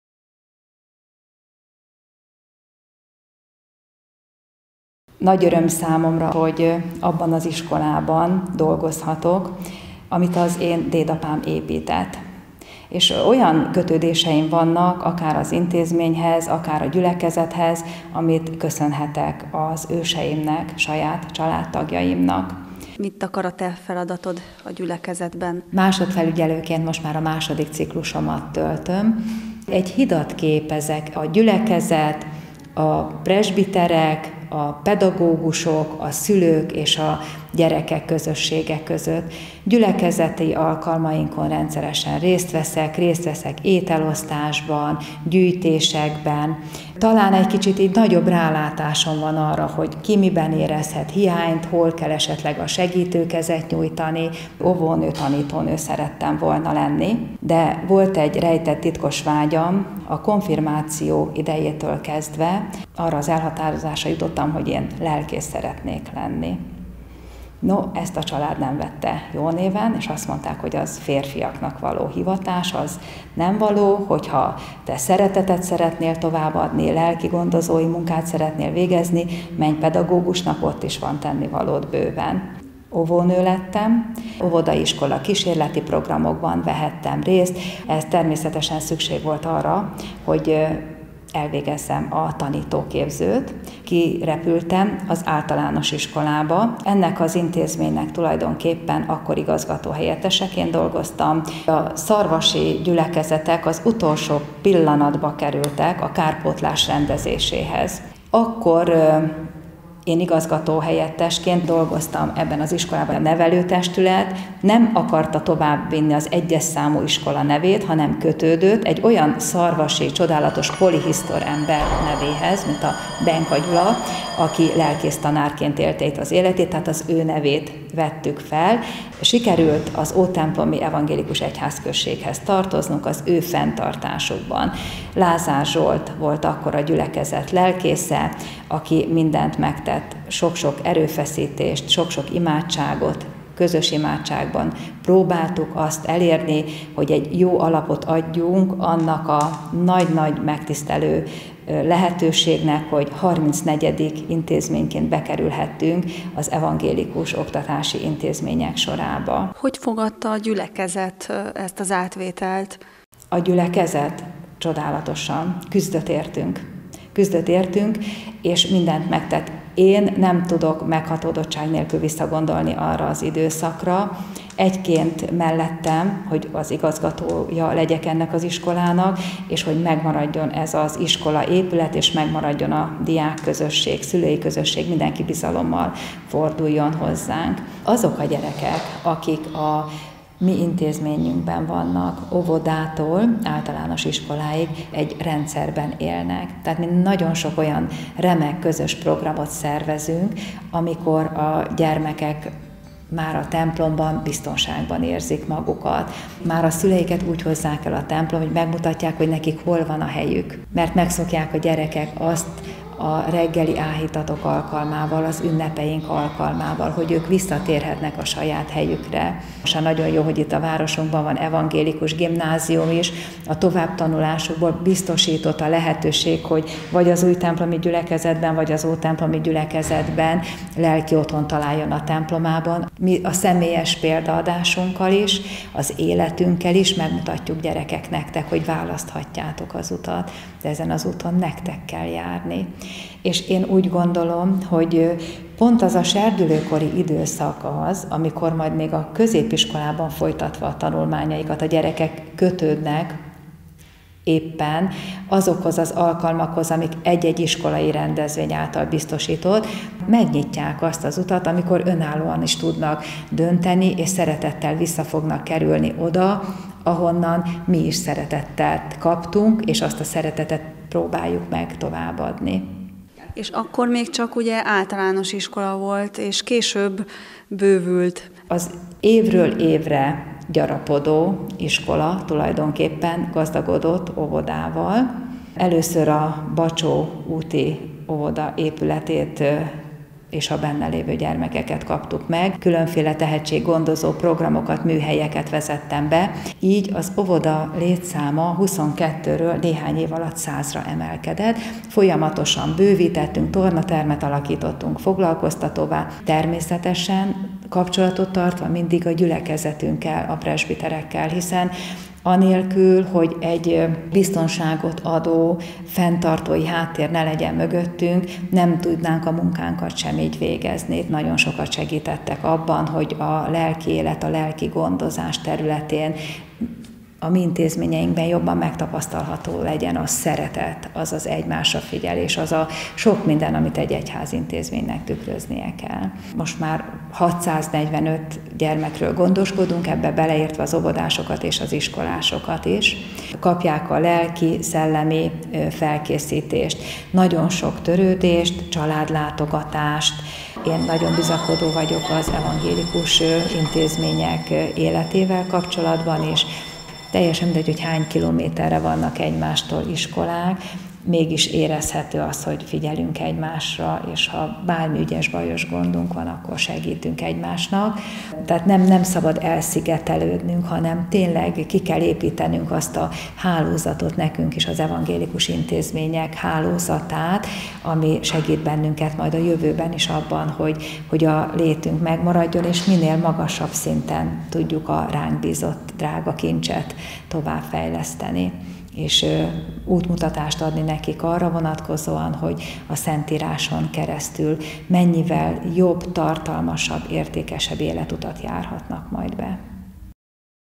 Gyülekezet és iskola – Hosszabb verzió
Molnár Istvánné Zsuzsa a Benka Gyüla Evangélikus Óvoda és Iskola igazgatónője beszél az intézményről és a helyi gyülekezethez való kapcsolatáról.

Videó: Győri András Timótheus, riporter: Horváth-Bolla Zsuzsanna